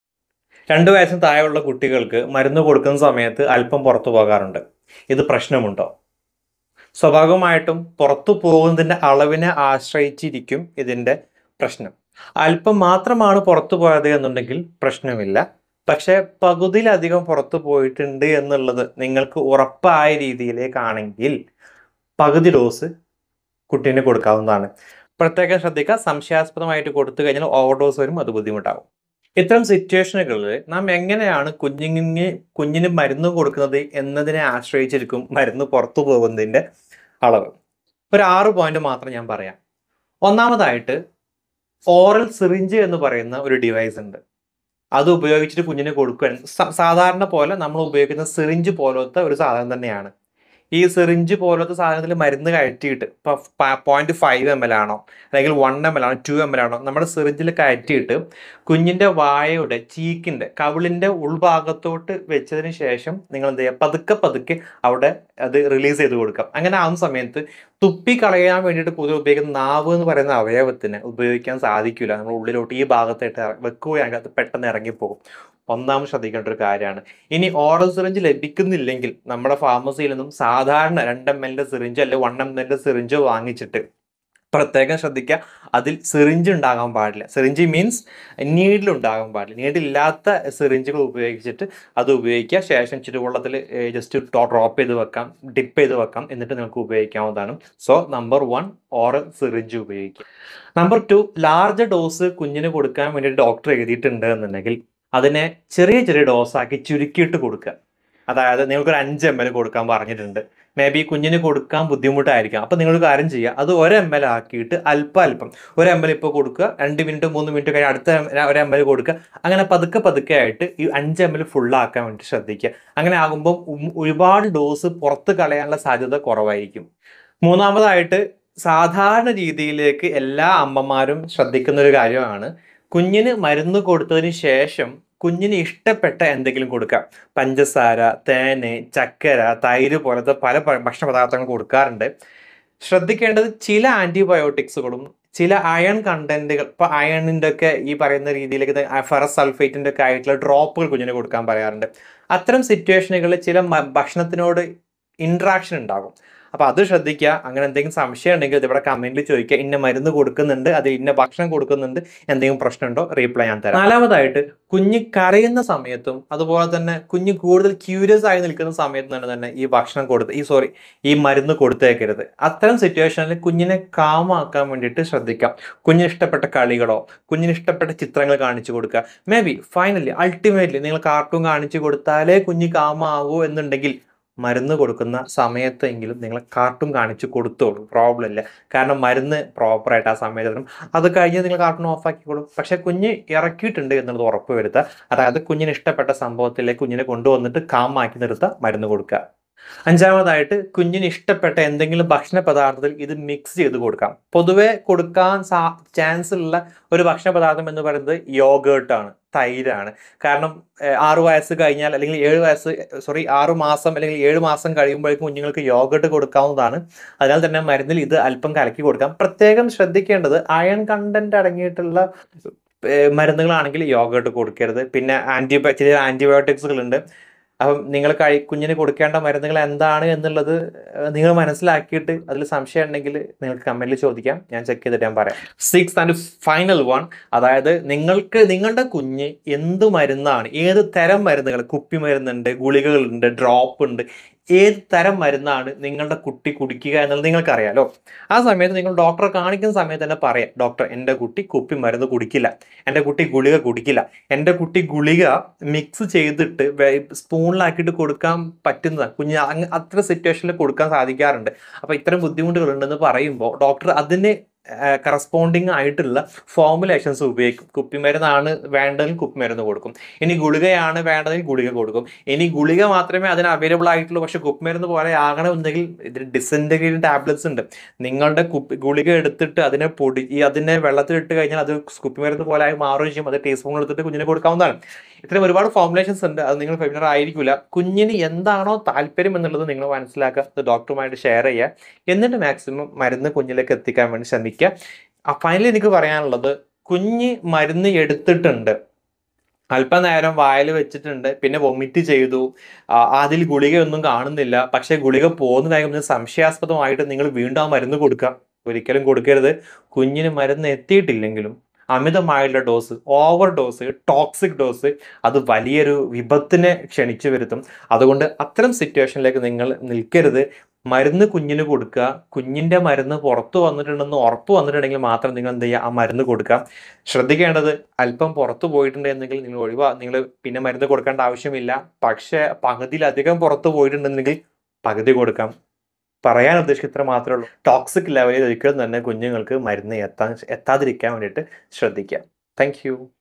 കുട്ടികൾക്കുള്ള മരുന്നിൽ 6 കാര്യങ്ങൾ ശ്രദ്ധിക്കണം | Tips for Baby Medicine |Pharmacy Doctor Talk
Giving medicine to kids can be tricky, but with the right approach, it becomes much easier and safer. In this video, I’ll share 6 practical tips to ensure your child takes their medication correctly and stress-free!
Watch now for detailed explanations of these tips and how to make medicine time easier for your little ones!